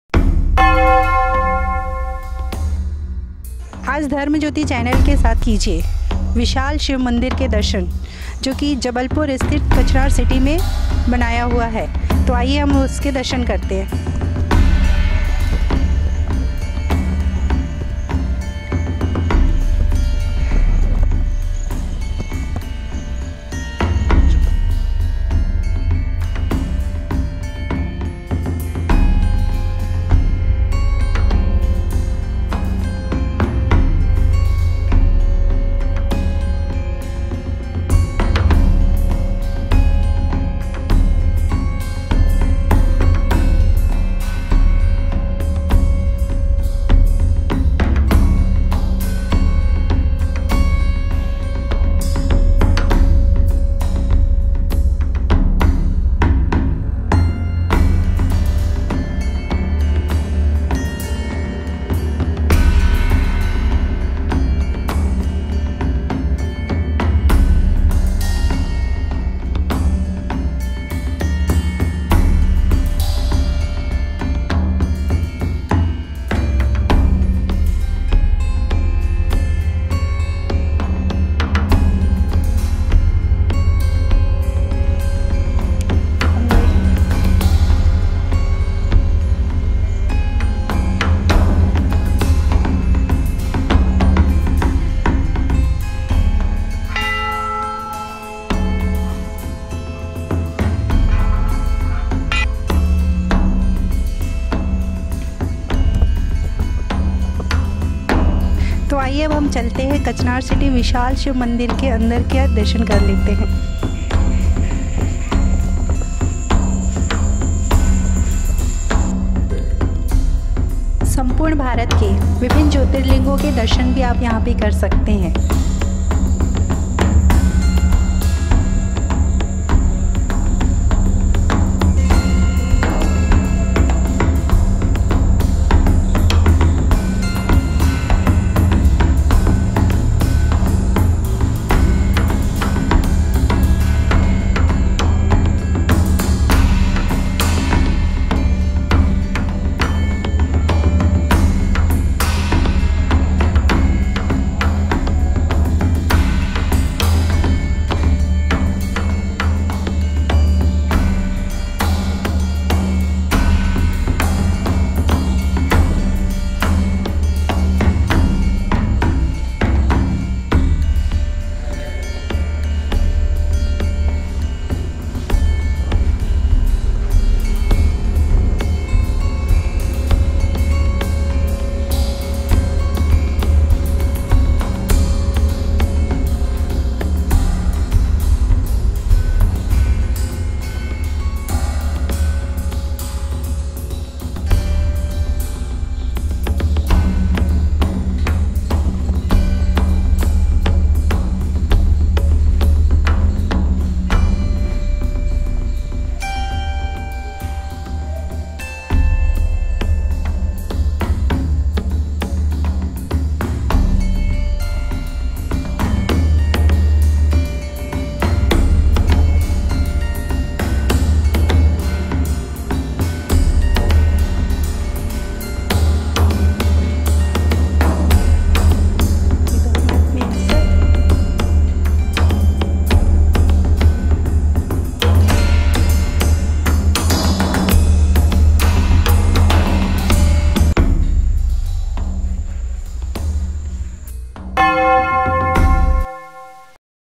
Dharmajyoti - Vidhee Asrani Visits world famous Shiv temple #@ Kachnar city Jabalpur
Editor-in-Chief of Dharmajyoti Channel,Vidhee Asrani Visits world famous Shiv temple @ Kachnar city Jabalpur